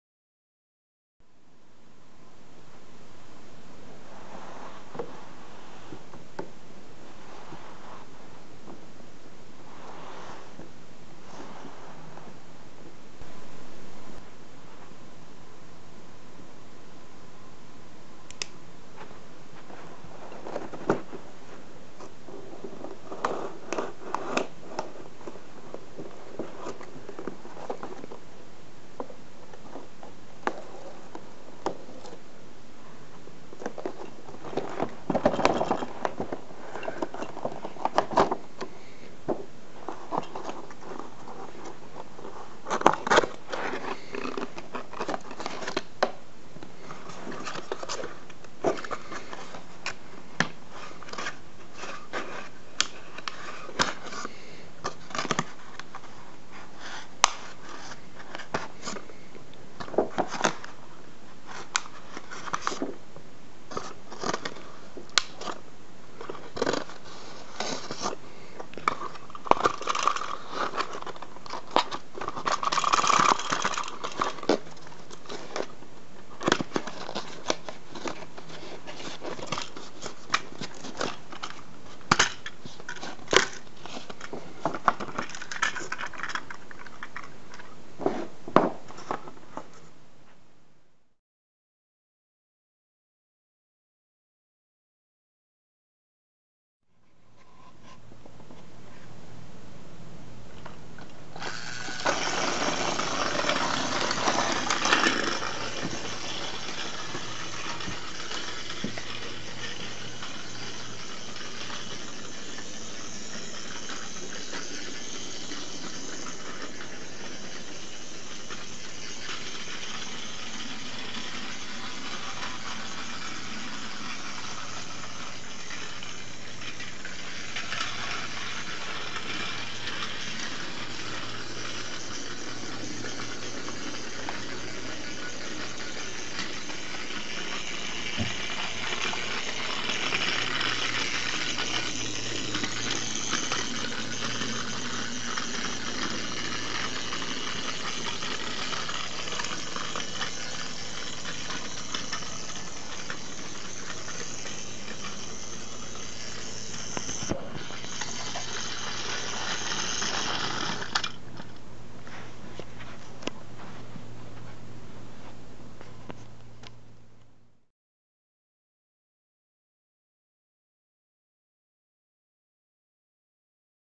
Trackmaster Iron Bert unboxing review & run
I got the Trackmaster Bert from Wal-Mart along with a Trackmaster Talking Percy a few days ago. I didn't felt like doing a unboxing review on it but I did because, when I've gotten Arry, I didn't have Bert but now I do.